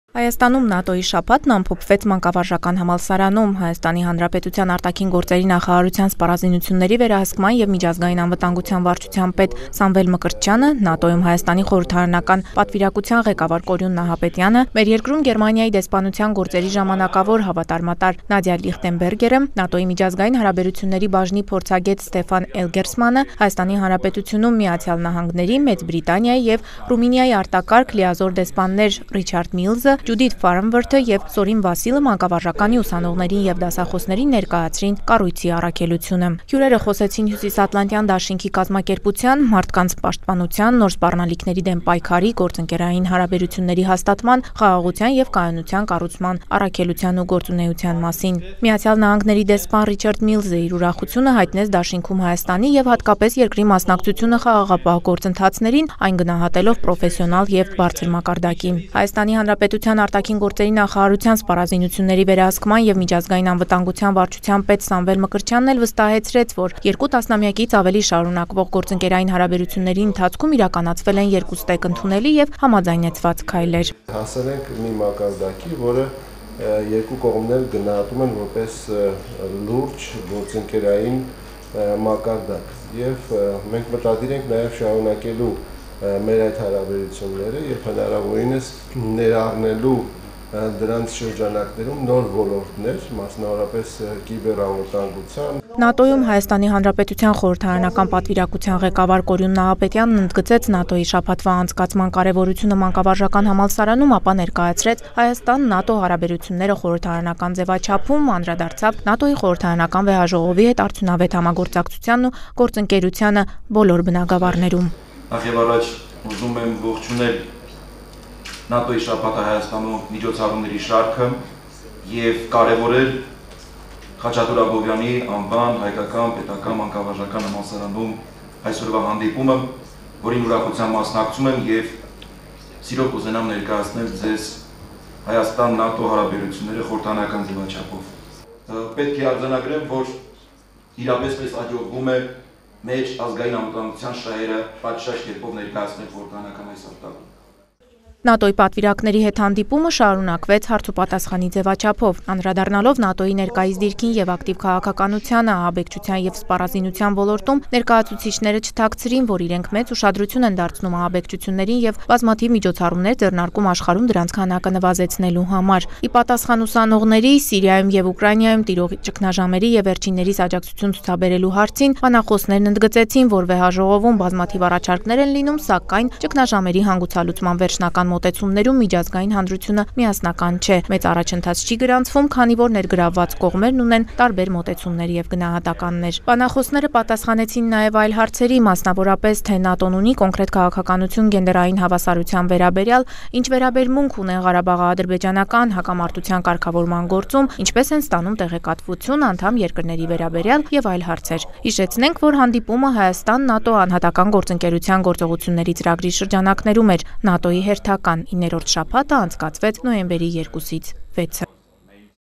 Հանդիպում ՆԱՏՕ-ի շաբաթվա շրջանակում
«ՆԱՏՕ-ի շաբաթ» ծրագրի շրջանակում Մանկավարժական համալսարանում տեղի ունեցավ հանդիպում բուհի պրոֆեսորադասախոսական կազմի և ուսանողների հետ:Հանդիպումը կազմակերպվել էր Հայաստան-ՆԱՏՕ Անհատական գործընկերության գործողությունների ծրագրի շրջանակում նոյեմբերի 2-6-ը Հայաստանում անցկացվող հերթական՝ 9-րդ ՆԱՏՕ-ի շաբաթվա շրջանակում: Բուհի պրոֆեսորադասախոսական կազմի և ուսանողների հետ հանդիպման ընթացքում պատվիրակները ներկայացրին Հյուսիս-Ատլանտյան Դաշինքի կազմակերպության՝ մարդկանց պաշտպանության, նոր սպառնալիքների դեմ պայքարի, գործընկերային հարաբերությունների հաստատման, խաղաղության և կայունության կառուցման առաքելությունն ու գործունեությունը: ՆԱՏՕ-ի պատվիրակների հետ հանդիպումը շարունակվեց հարցուպատասխանի ձևաչափով: